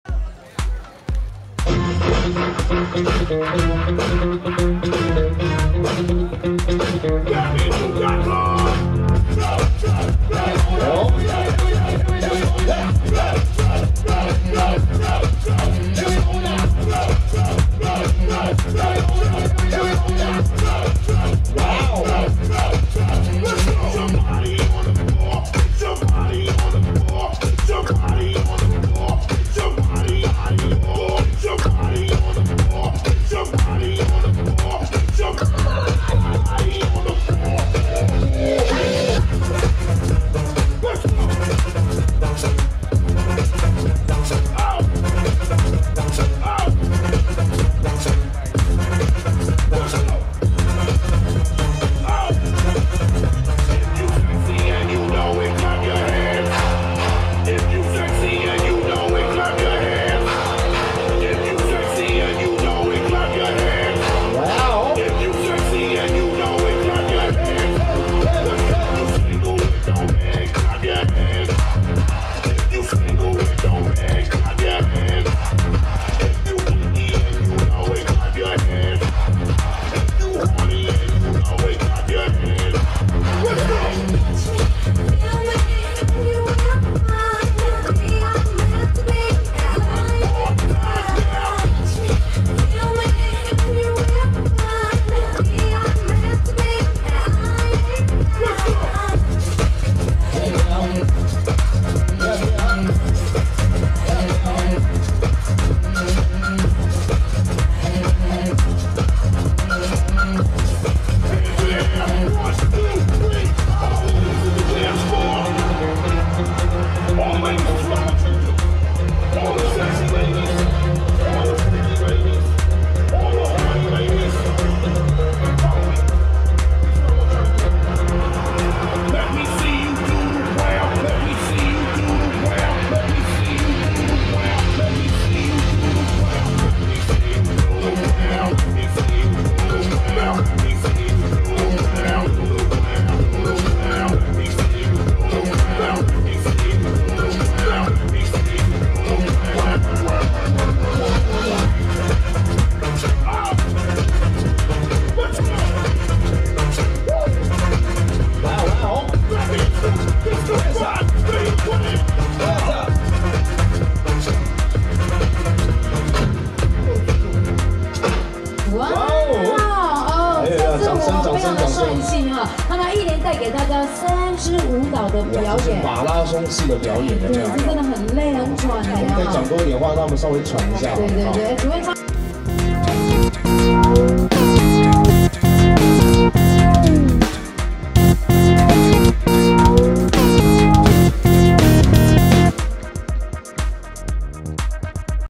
【無限HD】Baby Queens 熱舞19(4K HDR)@南州廣聖宮廣澤尊王創宮十週年往台南南勢街西羅殿進香回駕平安遶境【大港新聞】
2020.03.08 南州